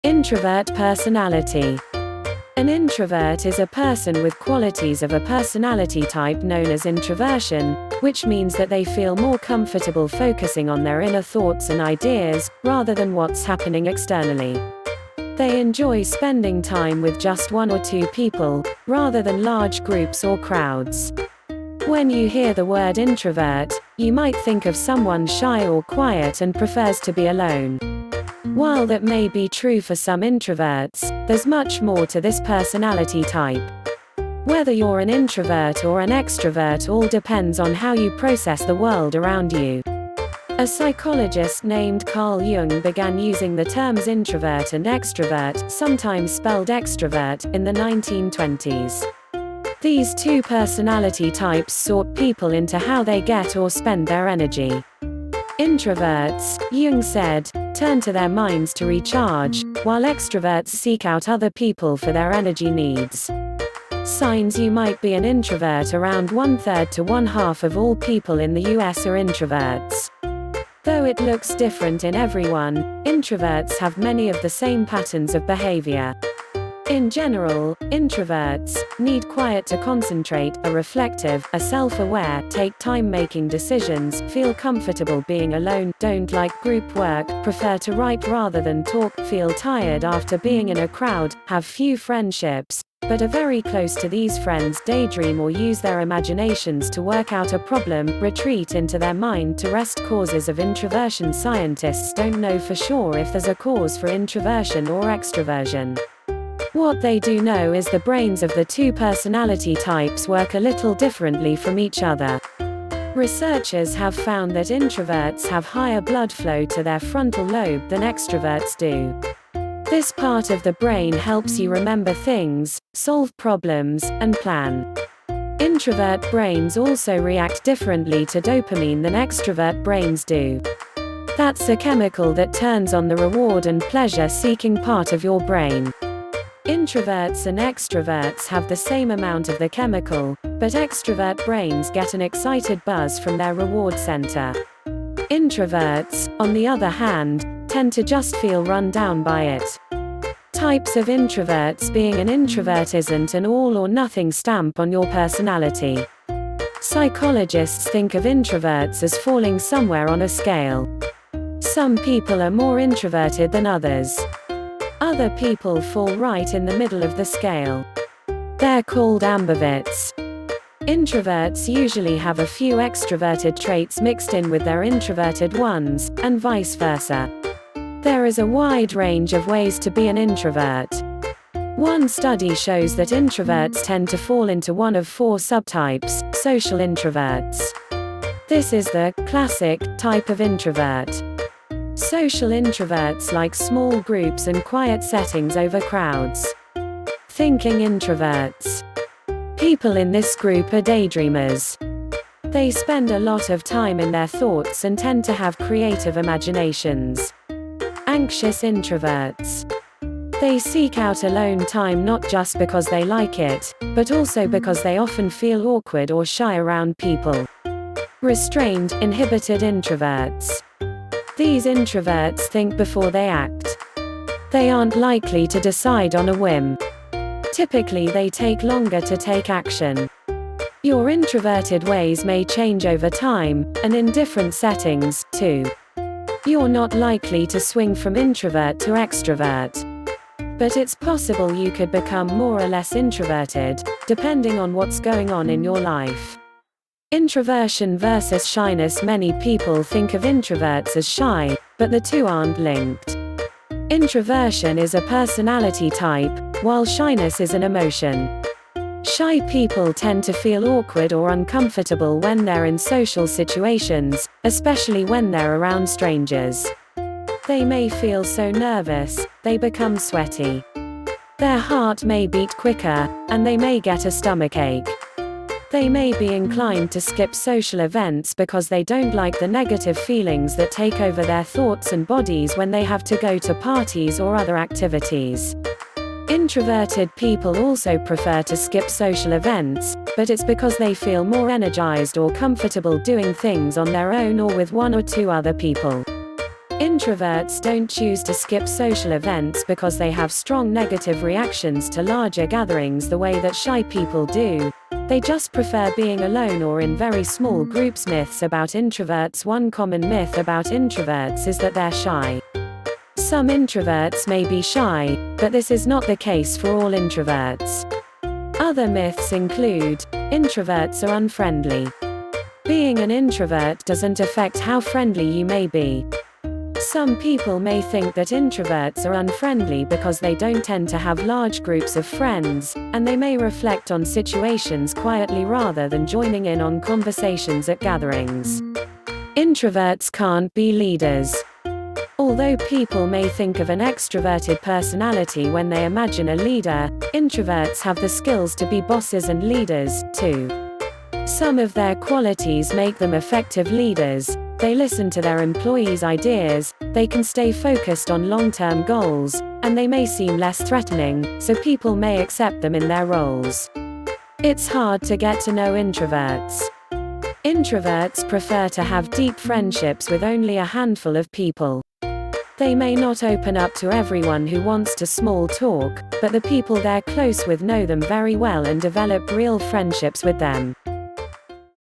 Introvert Personality | psycho leaks
An introvert is a person with qualities of a personality type known as introversion, which means that they feel more comfortable focusing on their inner thoughts and ideas, rather than what’s happening externally. They enjoy spending time with just one or two people, rather than large groups or crowds.
When you hear the word introvert, you might think of someone shy or quiet and prefers to be alone. While that may be true for some introverts, there's much more to this personality type. Whether you're an introvert or an extrovert all depends on how you process the world around you.